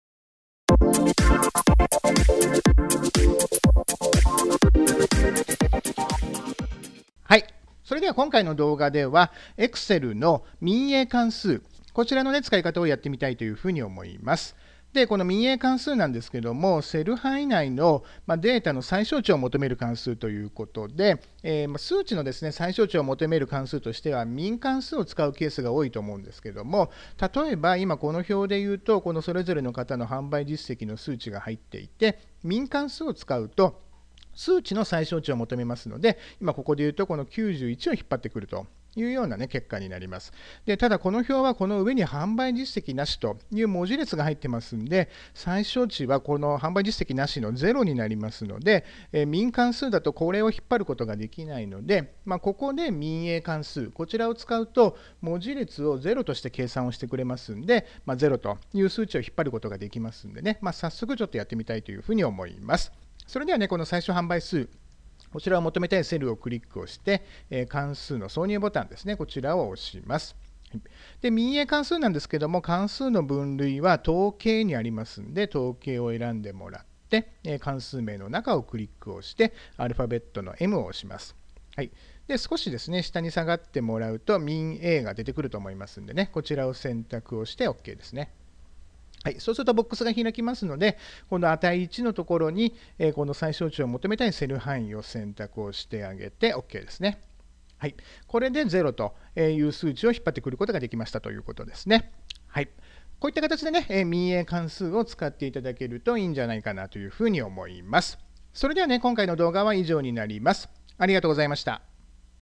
Excel MINA関数の使い方
この動画では、Excelの関数の使い方をご紹介。今回は、「MINA関数」の使い方のご紹介です。ぜひ、Excelの学習にお役立てくだ­さい。